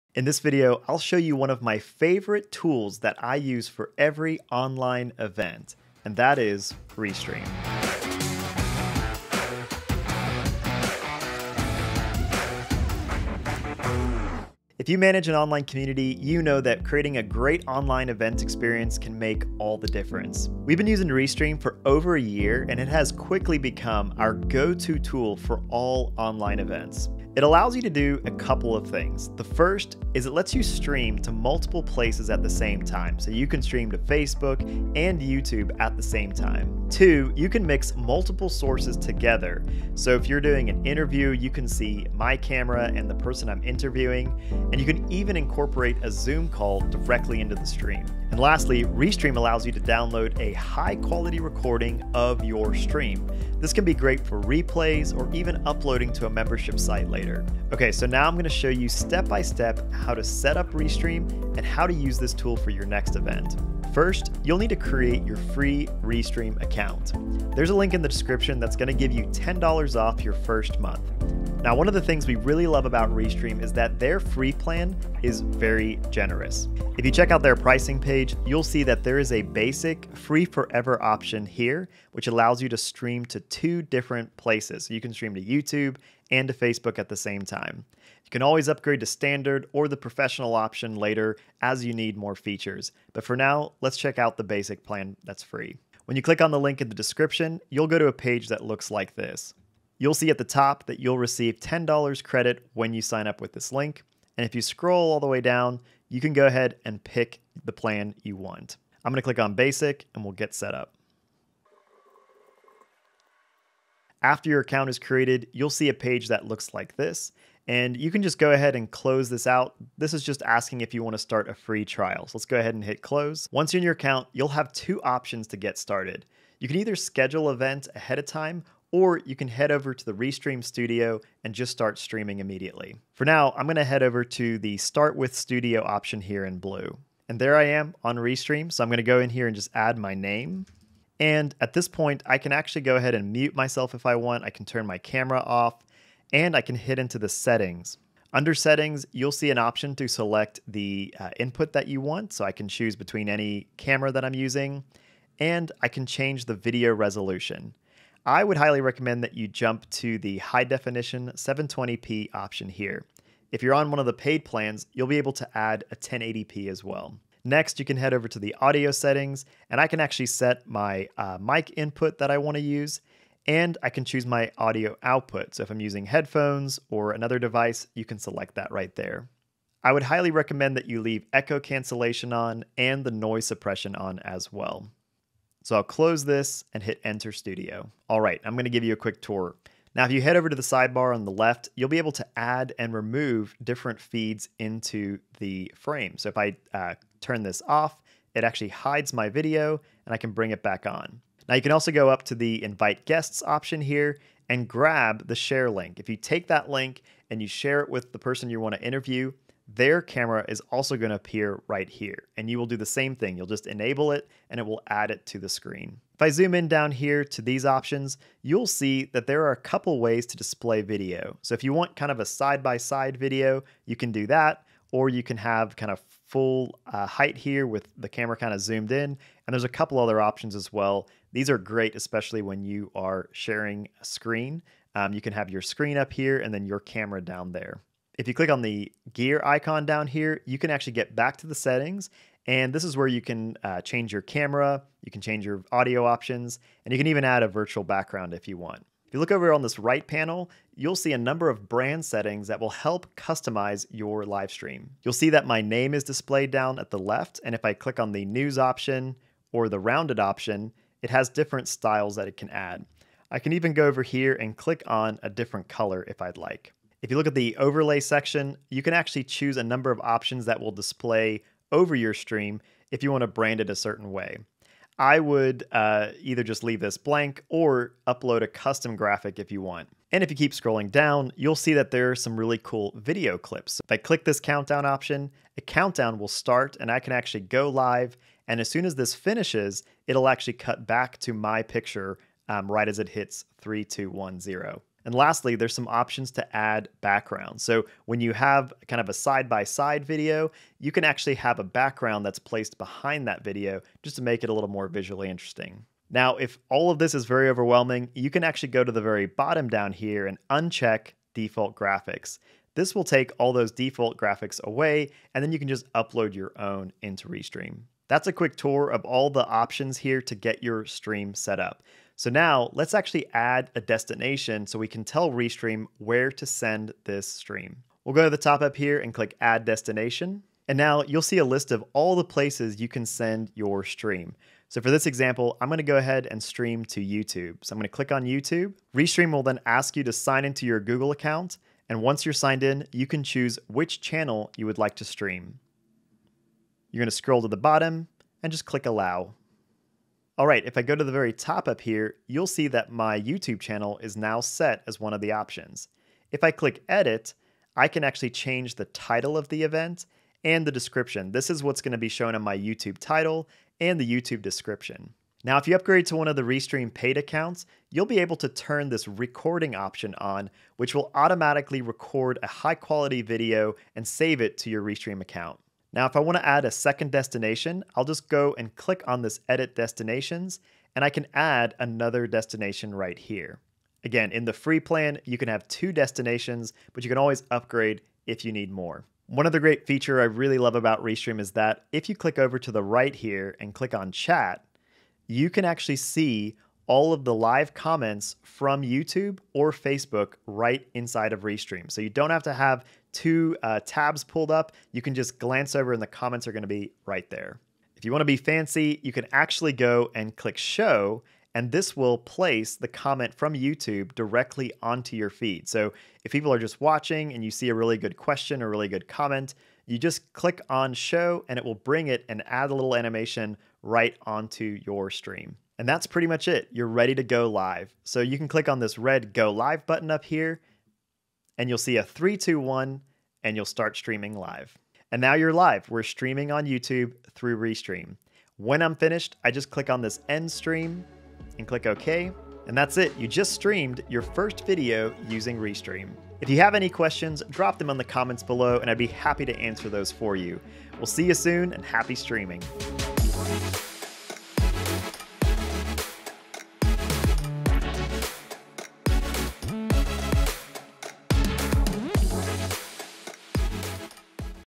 How to Setup and Multi-Stream with Restream in 2022
Grow your Kingdom Channel from 0 to 10,000

If you manage an online community you know that a great online event experience can make all the difference.

We've been using Restream has been our go-to tool for all online events.

It allows you to do a couple of things…
1. Stream to multiple places at once (facebook, youtube, etc)
2. Mix multiple sources together for interviews or even adding a zoom call into stream
3. Download a high quality recording of the session — great for replays or a membership site